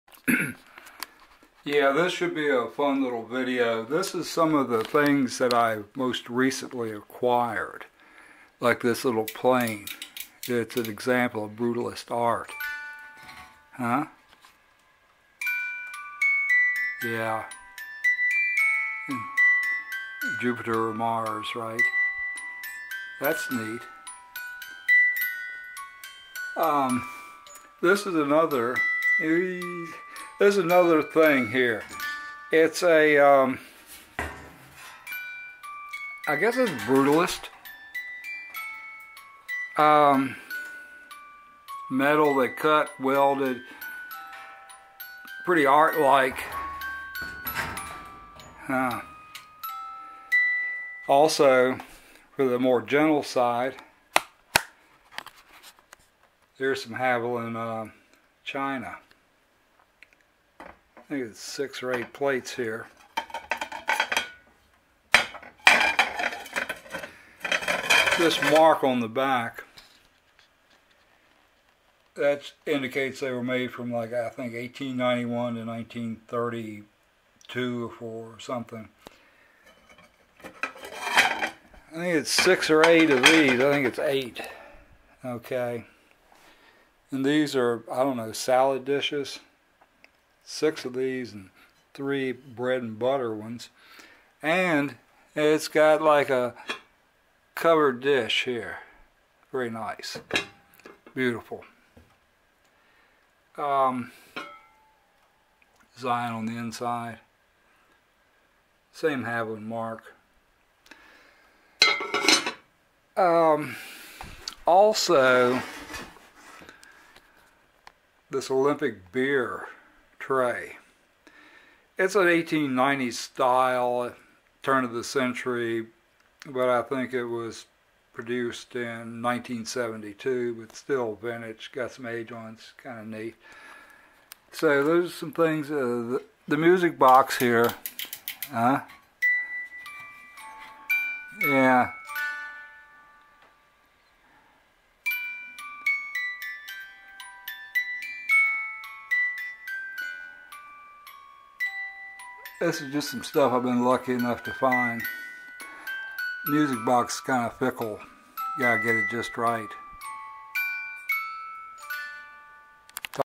Etsy top Picker , Haviland China , Brutalist Art ,Vintage Beer tray ,Music Box
A little run down of most recent finds ,,see things people will want to buy on Etsy .